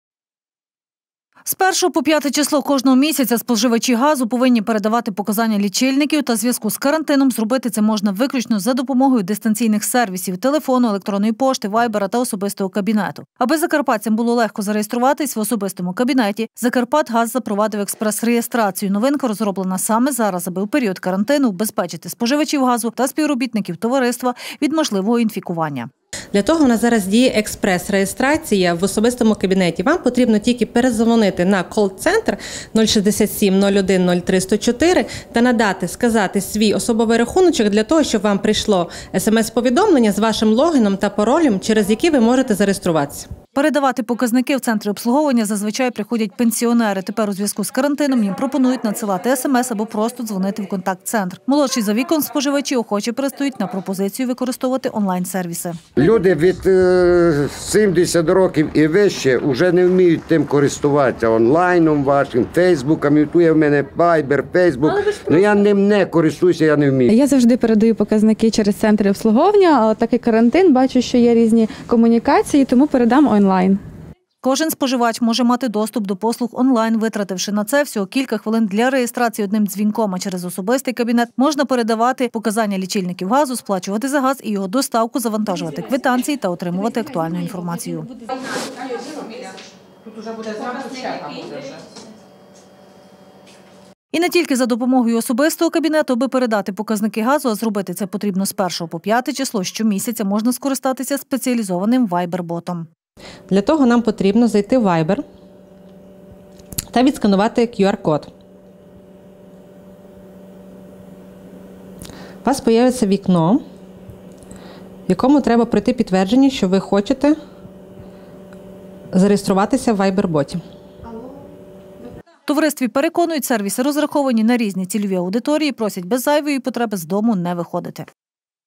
Показники в особистий кабінет
З 1 по 5 число кожного місяця споживачі газу повинні передавати показання лічильників. Та у зв’язку із карантином зробити це можна виключно за допомогою дистанційних сервісів: телефону, електронної пошти, вайбера та “Особистого кабінету”. Аби закарпатцям було легко зареєструватися в “Особистому кабінеті”, Закарпатгаз запровадив експрес-реєстрацію.  Новинка розроблена саме зараз – аби в період карантину убезпечити споживачів газу та співробітників товариства від можливого інфікування коронавірусною інфекцією.